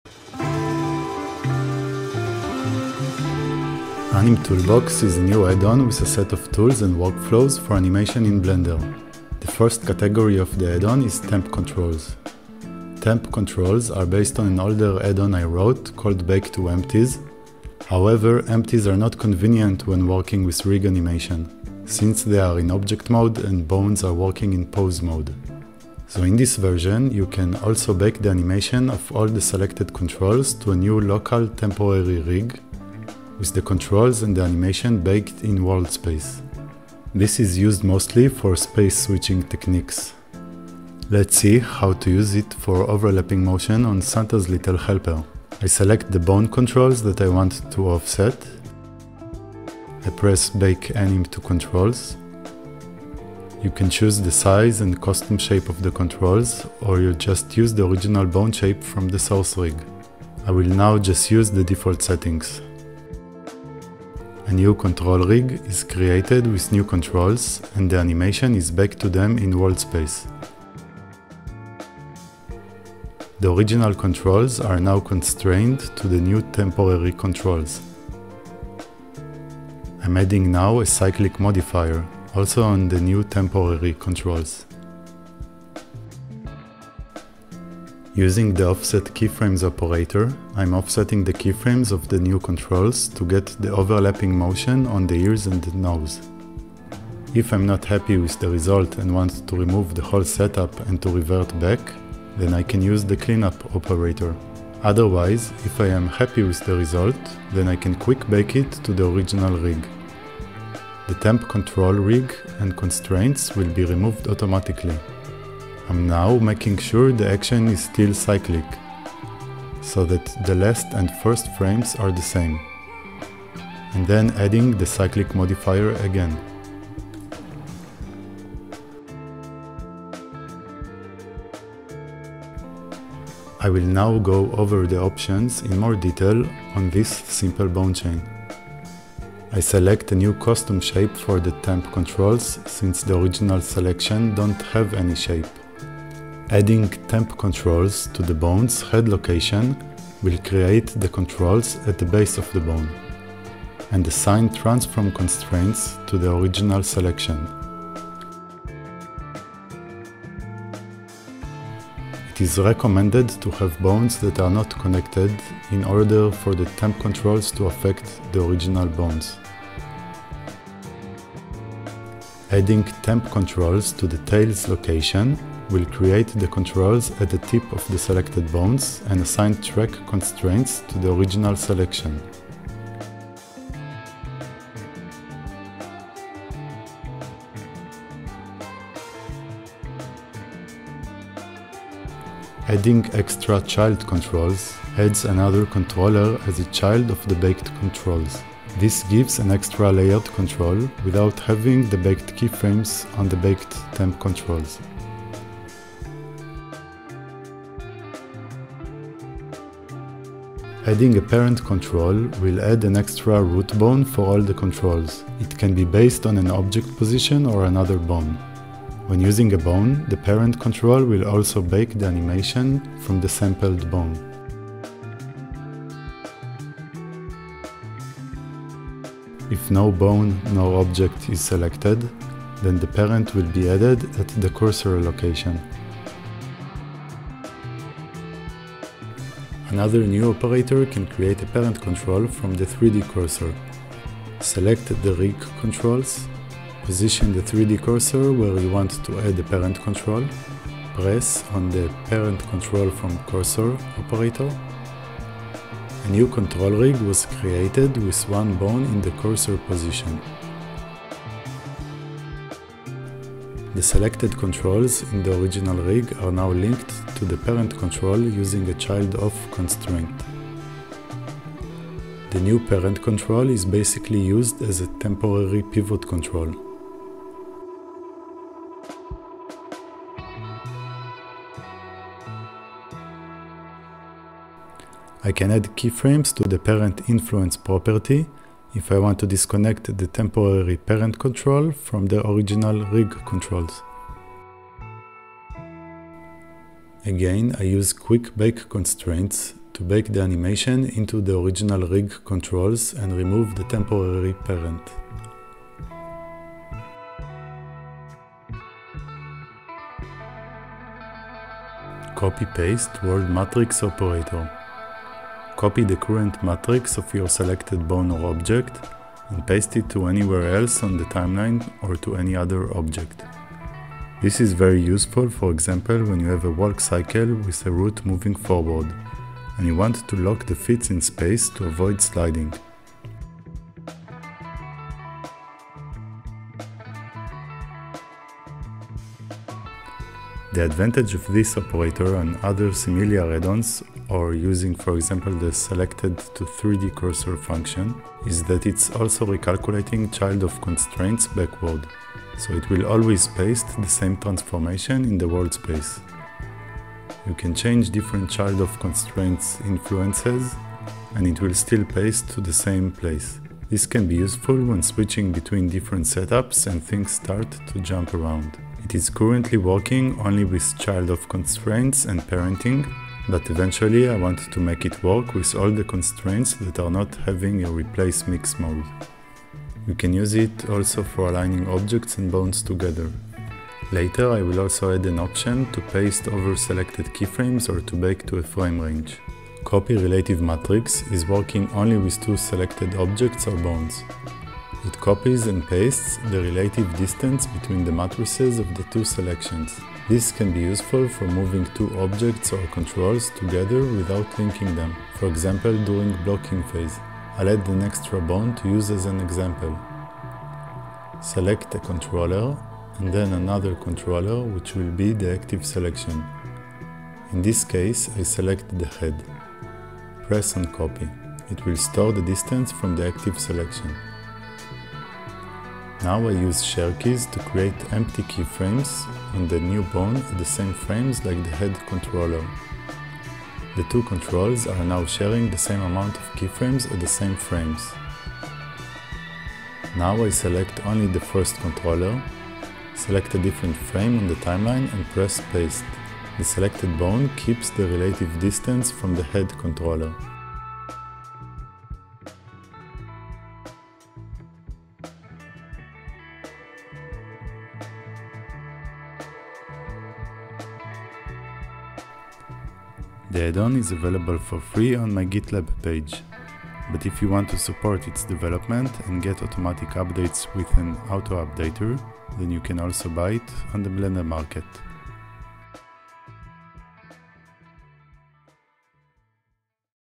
AnimToolbox Overview
Overview of all the current tools in Animtoolbox V0.0.0.3

00:10 - Temp Controls, Bake to Ctrls
00:58 - Bake Anim to Ctrls
01:28 - Offset keyframes
01:37 - Cleanup
01:45  - Quick Bake Constraints
02:26 - Temp Controls on the bone's Head Location
02:51 - Temp Controls on the bone's Tail Location
03:14 - Extra child Controls
03:29 - Parent/Root Control
04:00 - Parent Control on Cursor
05:12 - Copy World Matrix
05:40 - Copy World Matrix advantage - recalculating child of constraints
06:20 - Aligning objects
06:29 - Copy Relative Matrix
07:05 - Share Keyframes